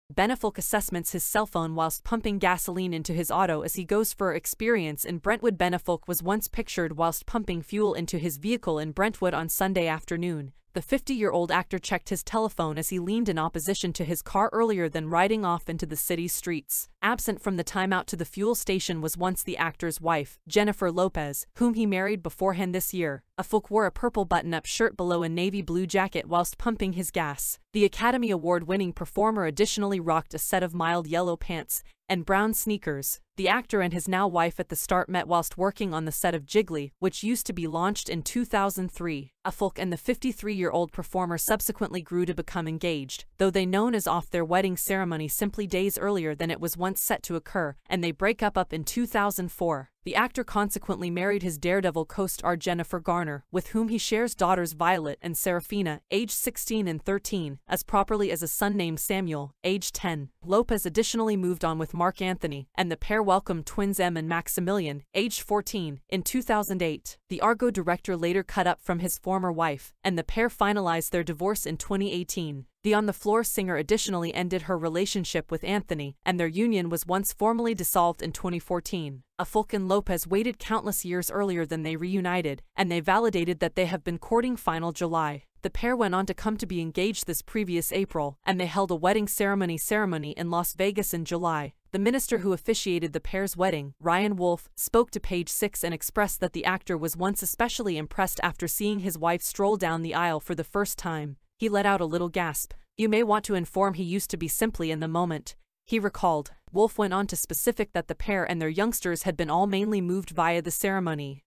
Ben Affleck checks his phone while filling up his car
Ben Affleck assesses his cellphone whilst pumping gasoline into his auto as he goes for an experience in Brentwood Ben Affleck was once pictured whilst pumping fuel into his vehicle in Brentwood on Sunday afternoon.